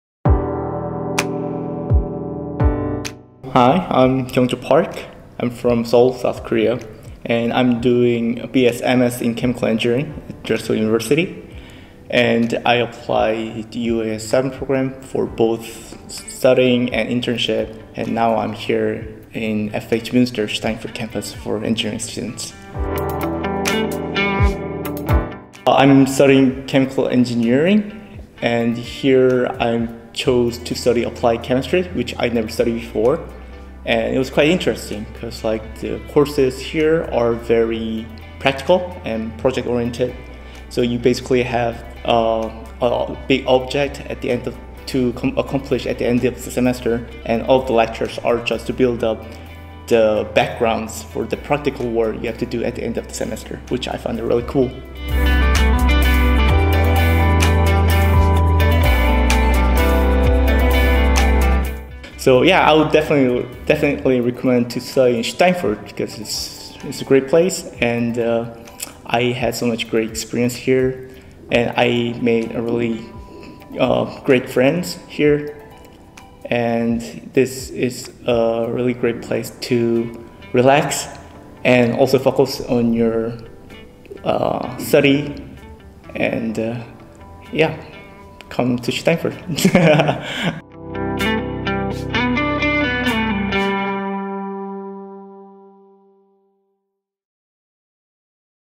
Kyeongjo Park I Study & Internship Program I FH Münster
Kyeongjo Park, a South Korean Chemical Engineering student at Drexel University, participates in the UAS7 Study and Internship Program at FH Münster University of Applied Sciences. In this video, he speaks about his experience studying and interning in Germany.